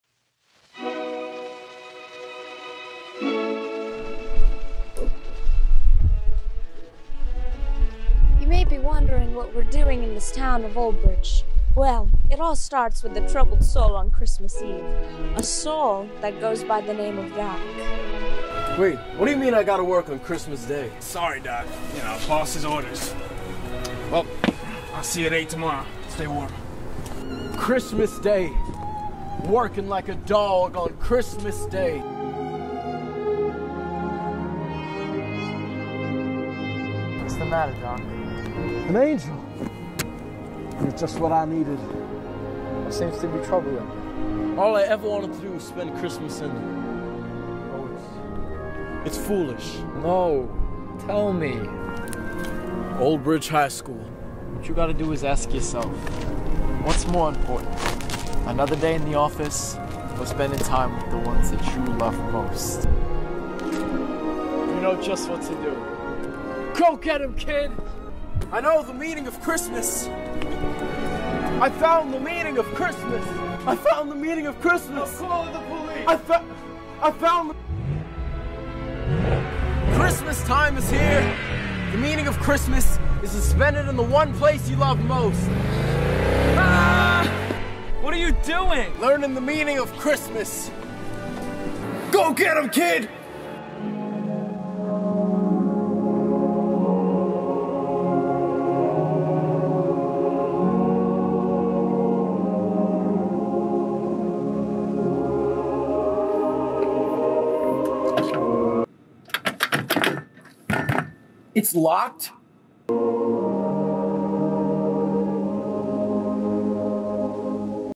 It’s a wonderful knight- short film
Directed and audio by Matthew Wrate
Directed and cinematography by Kailey Siegel
Acted by Matthew Wrate, Kailey Siegel, Nathan Pitts and Ryan ArBeeny